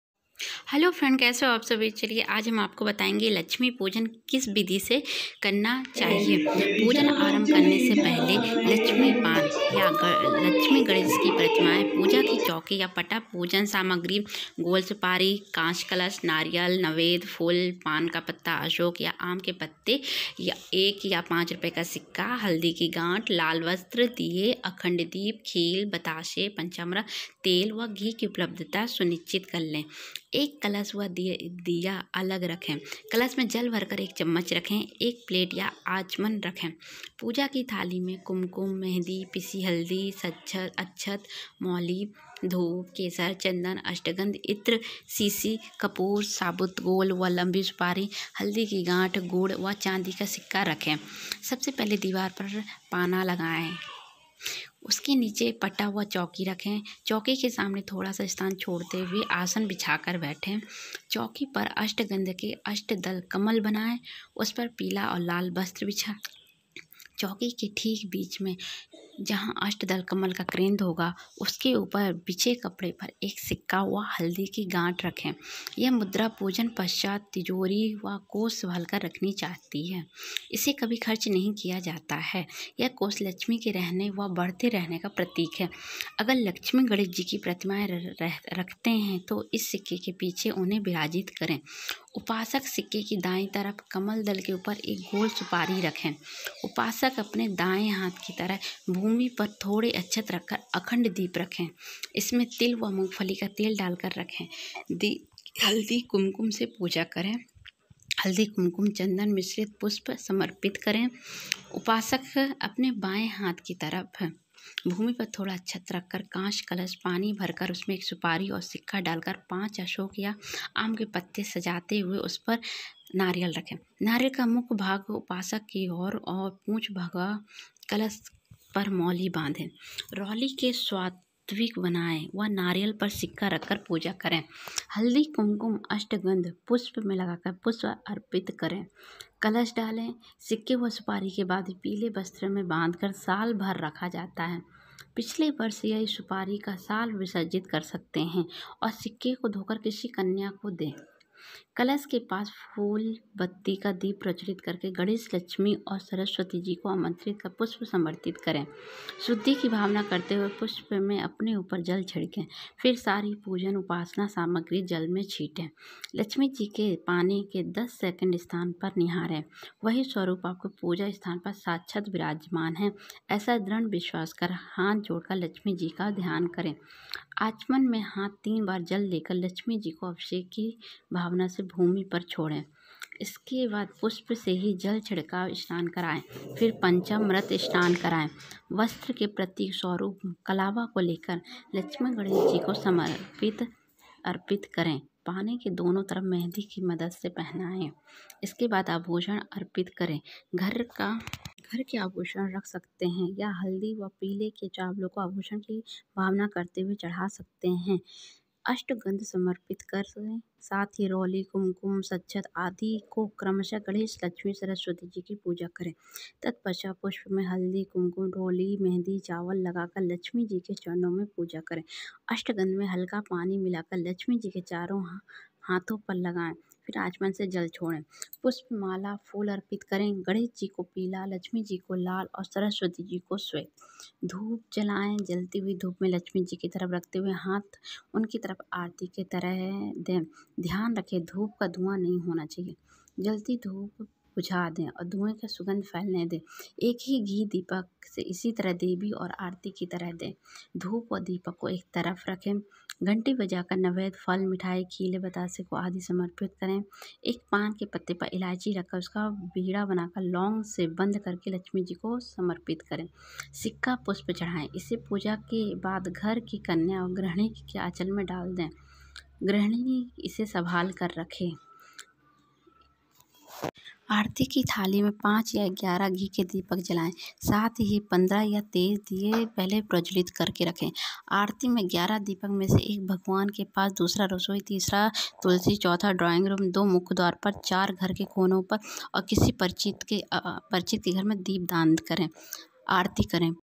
दीवाली में लक्ष्मी जी पूजा कैसे करे सरल विधिdiwali puja samagri#bidhi#beautiful gyan
lakshmi ji puja kaise ki jati h isme mene sb btaya h kaise ki jati h sabse pahle kya kiya jata h kaise kiya jata h jaise maine aapko vaise bidhi se krna chahiye